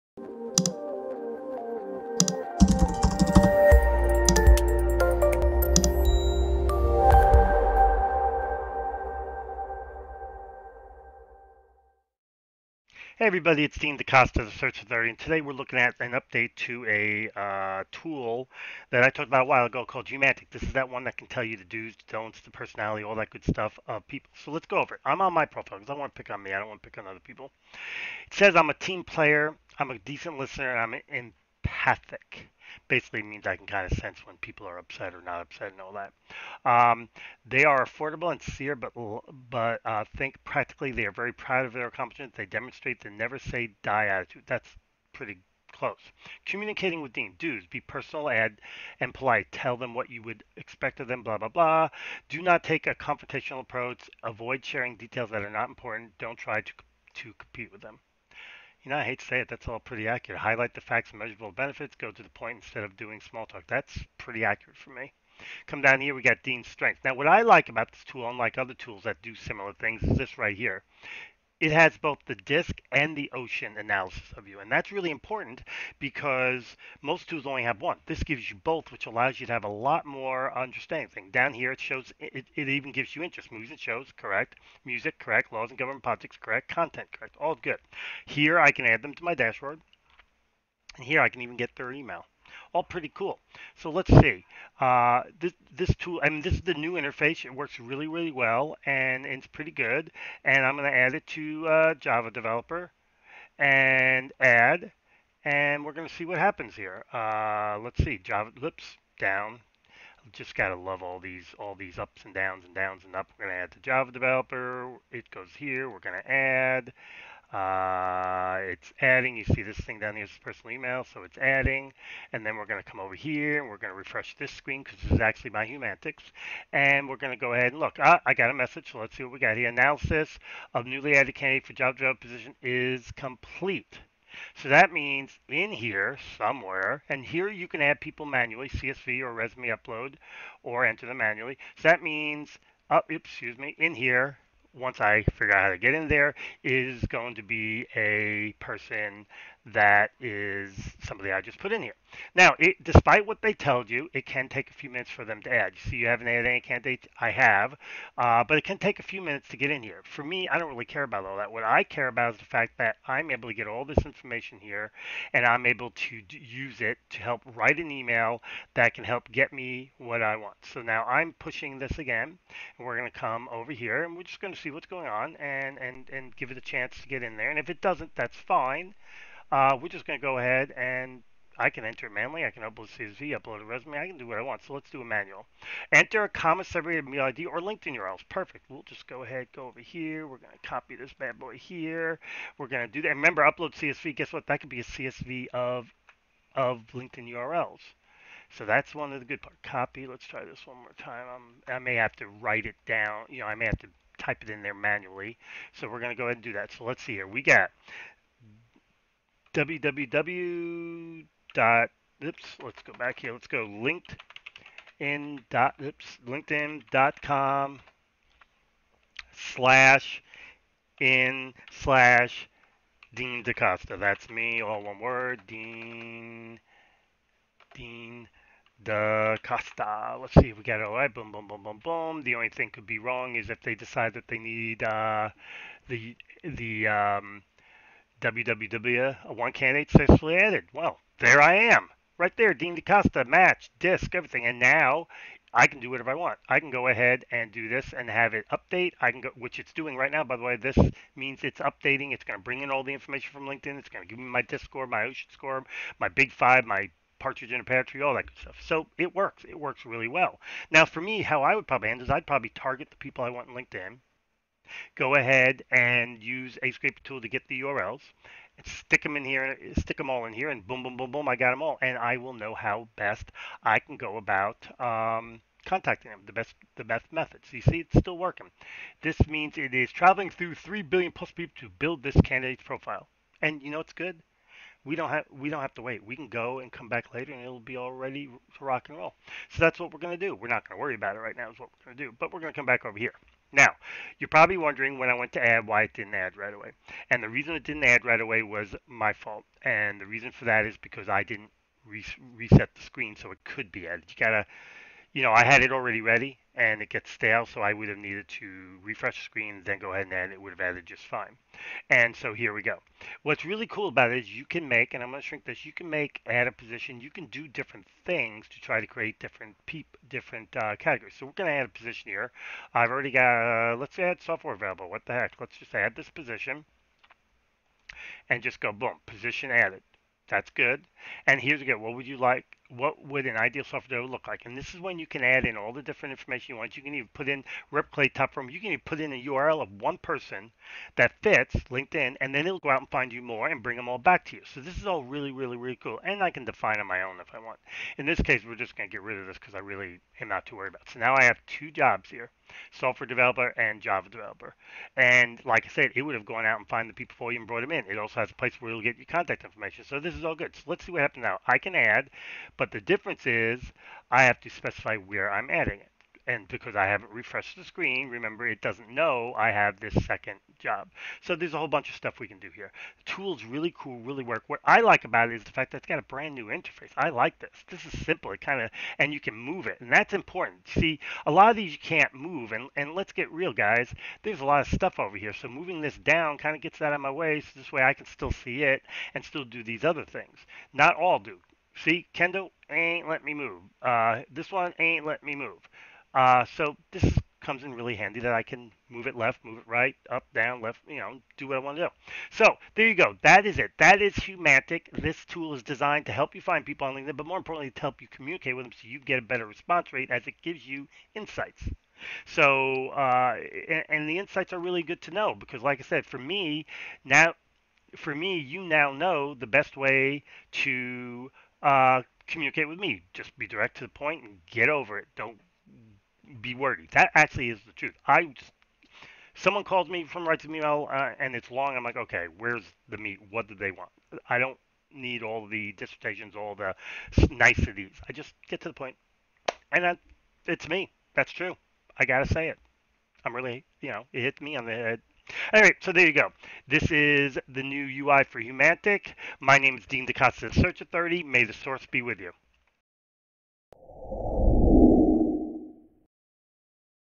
Humantic's New UI for Candidate Engagement and Assessment
Wouldn't it be great if you could get all of your candidates to take a personality test? It would certainly help you see if they are a good fit for your team. Well, now there's a way to get a candidate personality assessment without a test. Humantic AI is a tool for candidate engagement and assessment for recruiters and sales professionals. The tool facilitates understanding an individual's unique personality and learns how they make decisions.
Check out Dean's demo and then head over to humantic.ai to install their Chrome Extension/sign up for their waiting list!